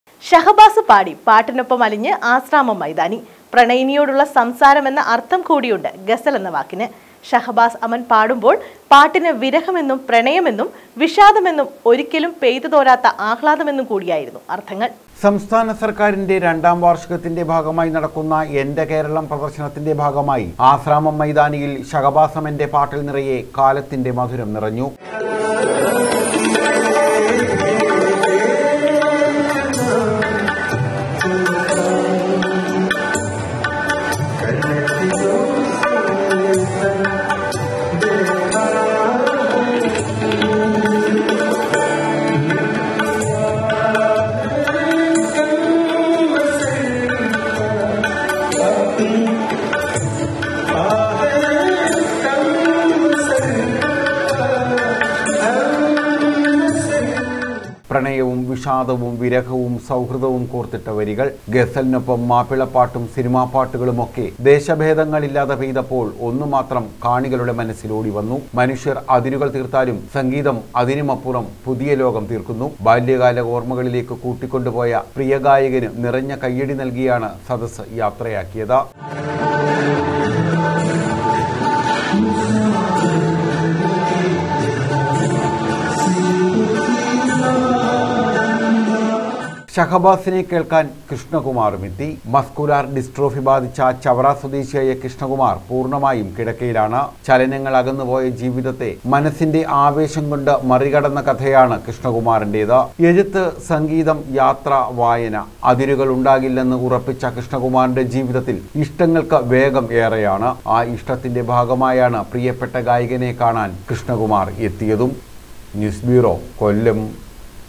ഷഹബാസ് പാടി, പാട്ടിനൊപ്പമലിഞ്ഞു ആശ്രാമം മൈതാനി
ഷഹബാസ് പാടി, പാട്ടിനൊപ്പമലിഞ്ഞു ആശ്രാമം മൈതാനി. പ്രണയിനിയോടുള്ള സംസാരമെന്ന അര്‍ഥം കൂടിയുണ്ട് ഗസലെന്ന വാക്കിന്.ഷഹബാസ് അമന്‍ പാടുമ്പോള്‍ പാട്ടിന്, വിരഹമെന്നും പ്രണയമെന്നും വിഷാദമെന്നും ഒരിക്കലും പെയ്തു തോരാത്ത ആഹ്ലാദമെന്നും കൂടിയായിരുന്നു അര്‍ഥങ്ങള്‍.